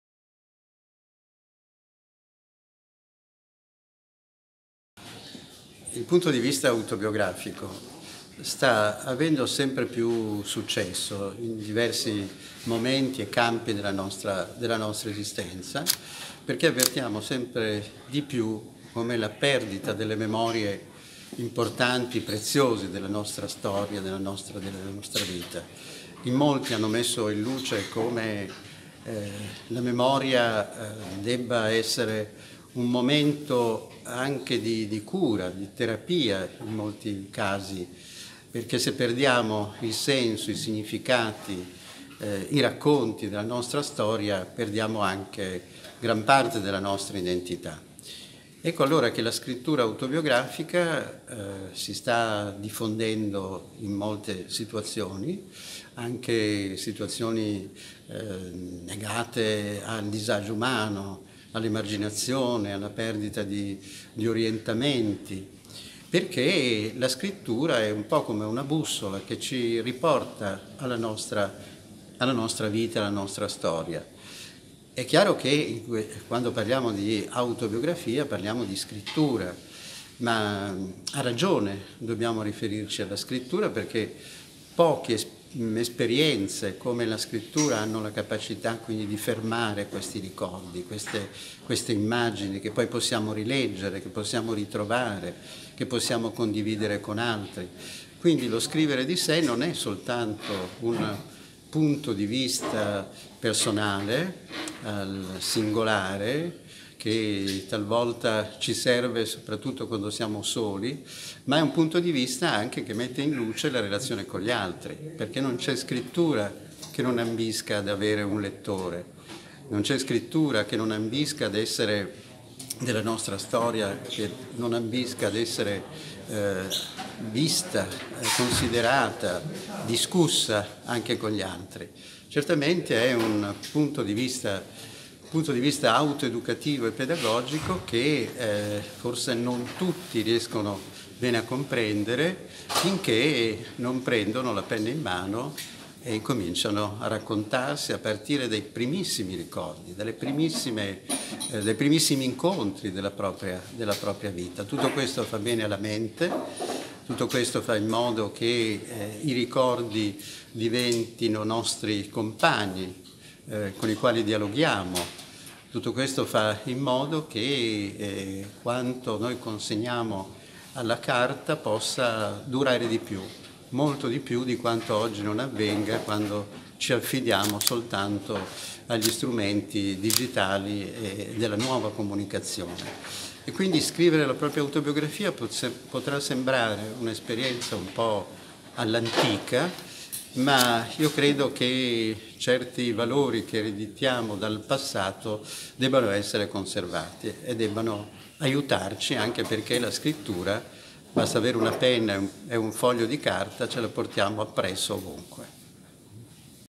Prof. Duccio Demetrio
Duccio Demetrio  ha animato i primi passi dell’incontro dei Missionari della Consolata prossimi ai 25 anni di ordinazione.